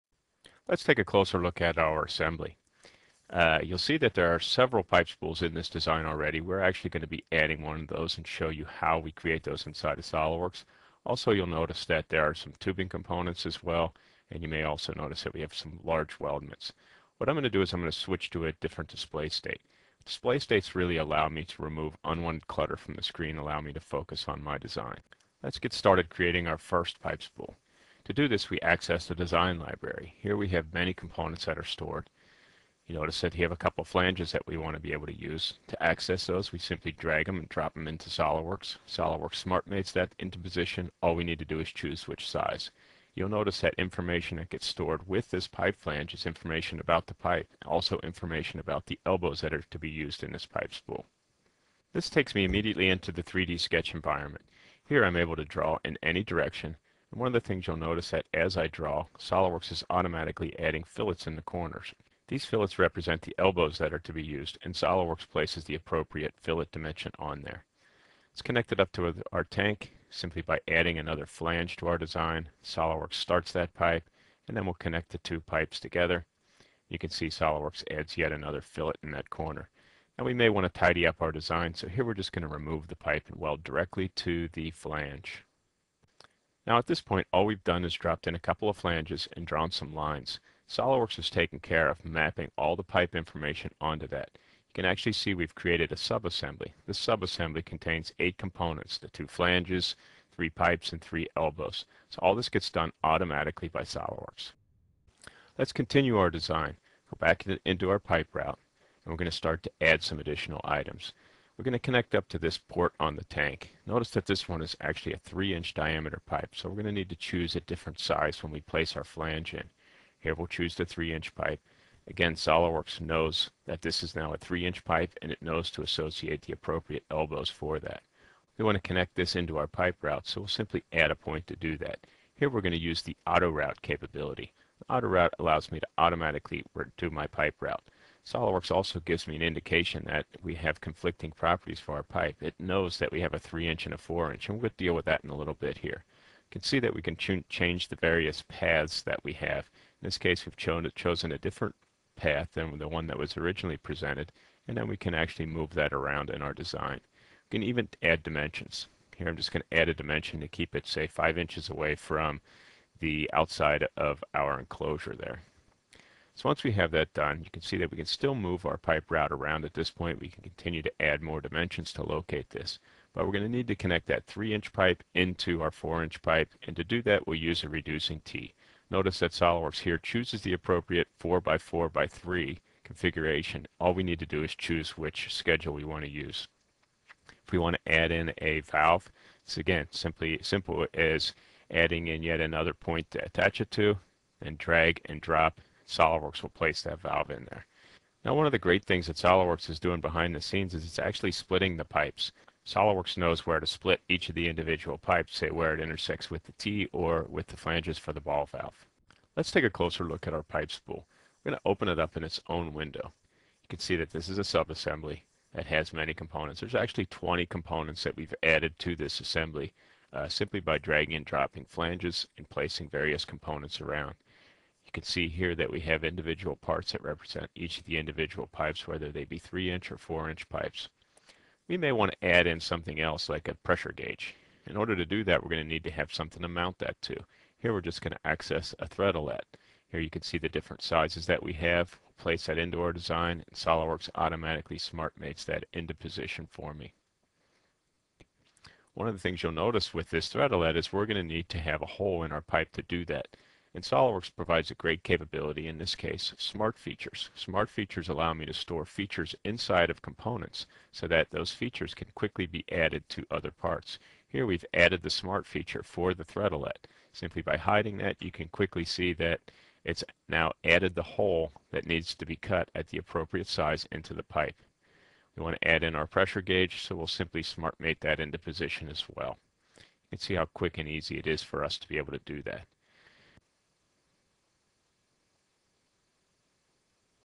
SolidWorks Routing
The Routing application is an add-in to the SolidWorks Premium software. With Routing, you can create a special type of subassembly that builds a path of pipes, tubes, or electrical cables between components